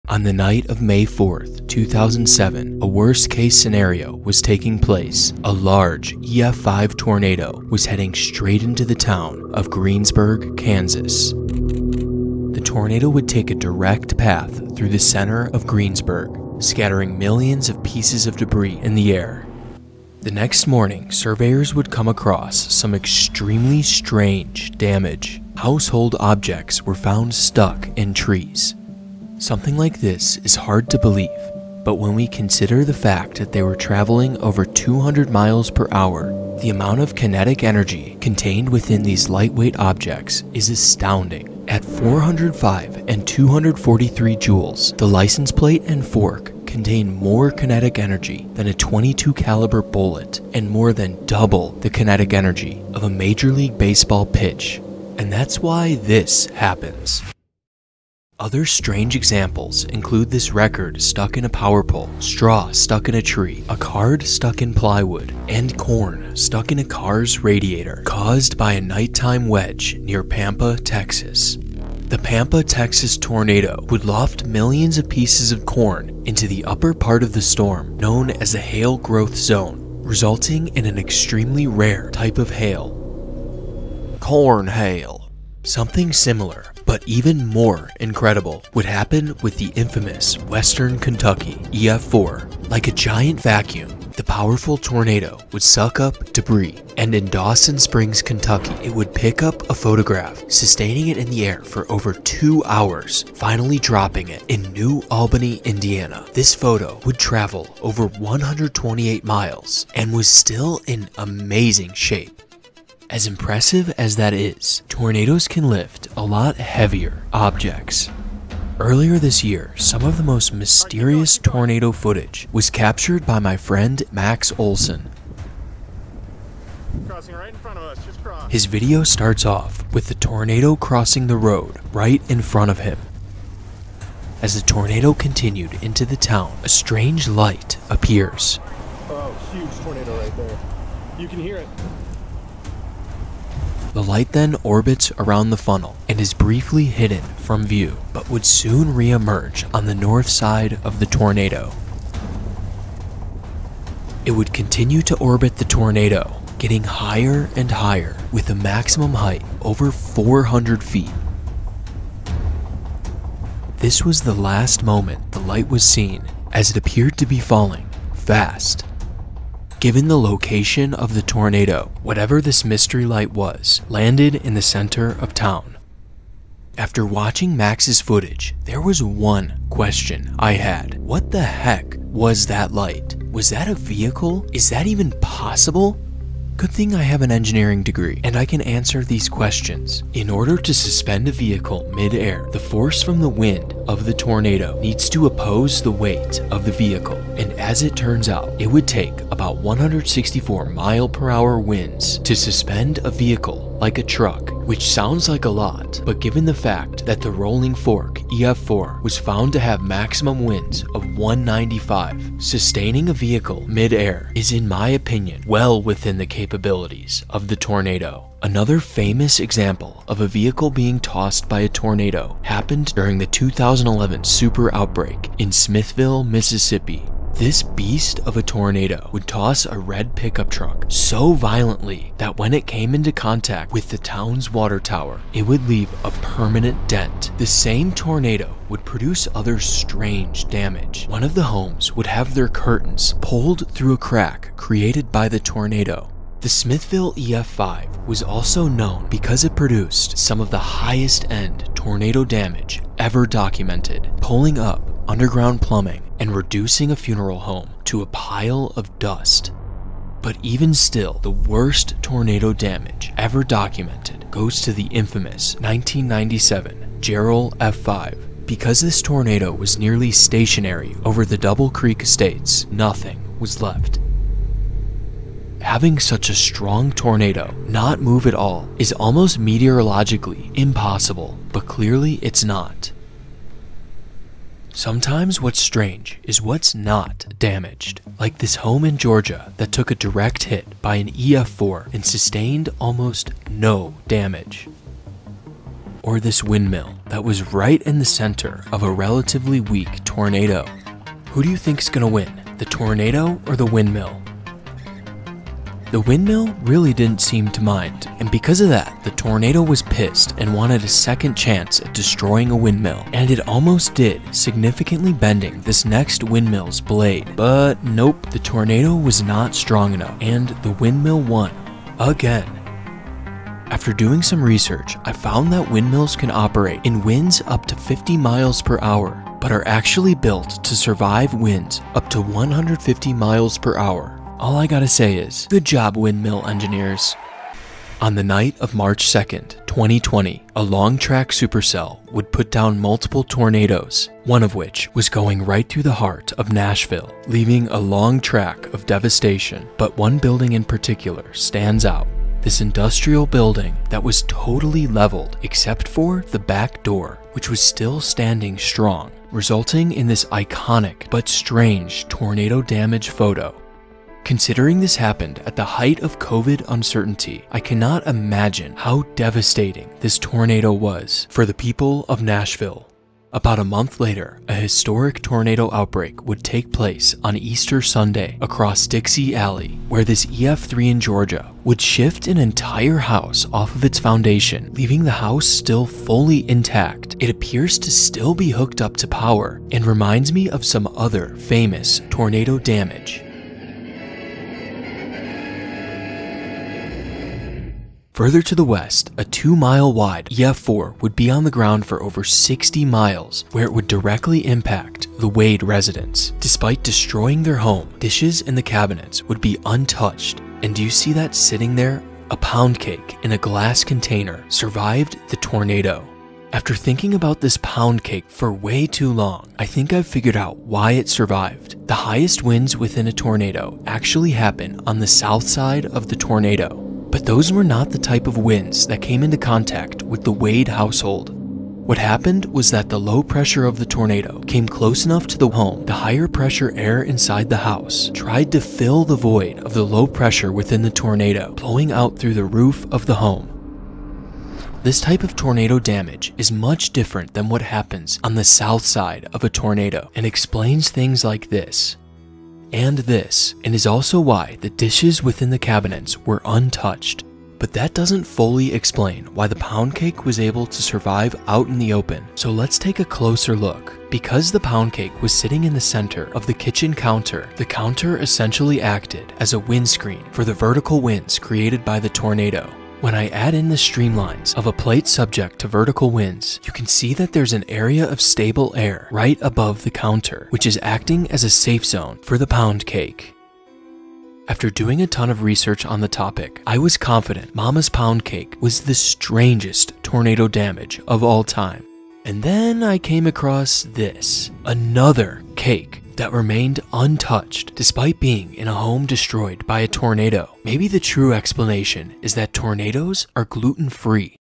The STRANGEST Tornado Damage Ever Documented
Tornadoes featured:
2007 Greensburg EF5
2015 Pampa EF3
2021 Mayfield EF4
2023 Rolling Fork EF4
2011 Smithville EF5
1997 Jarrell F5
2021 Crowell Texas Tornado
2020 Nashville EF3
2020 Bassfield EF4